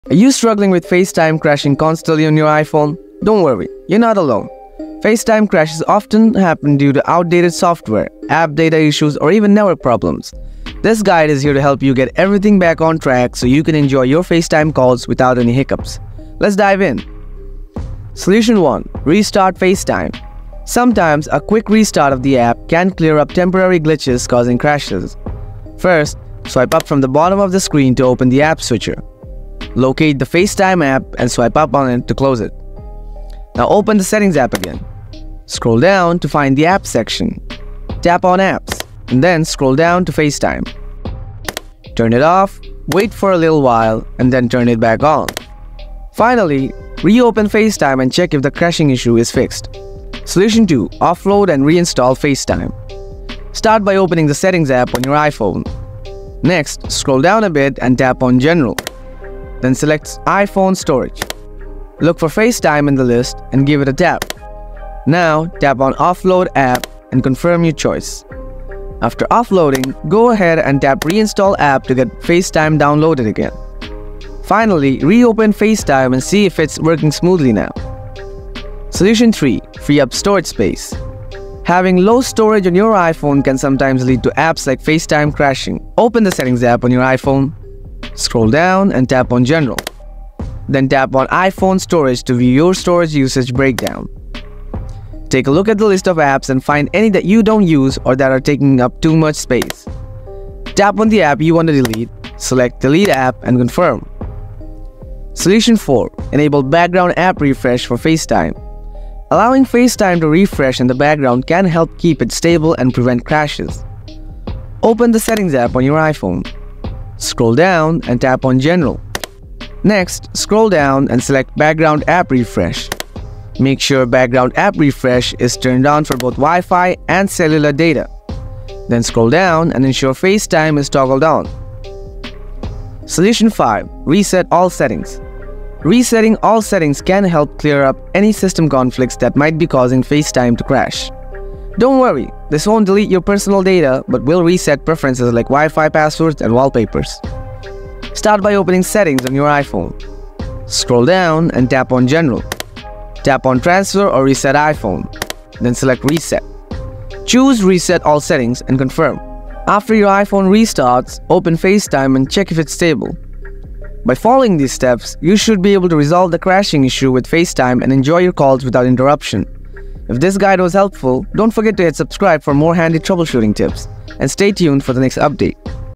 How To Fix Facetime App Keeps Crashing On iPhone | Solve Facetime Not Working on iPhone iOS 18
Does your iPhone Facetime keep freezing after an iOS 18 update? Or are you facing the Facetime closing on its own on your iPhone? Either way, this video will help you resolve the Facetime crash error and fix the unresponsive Facetime on your iPhone permanently!

It can be pretty frustrating when Facetime simply crashes when connecting to an iPhone call, so we should fix it right away! Just follow me along properly and you'll do that in a matter of minutes!

00:01- Video Intro
00:17- Restart FaceTime
00:50- Reinstall FaceTime
01:17- Free Up Storage
01:46- Background App Refresh
02:13- Reset All Settings
02:49- Solution End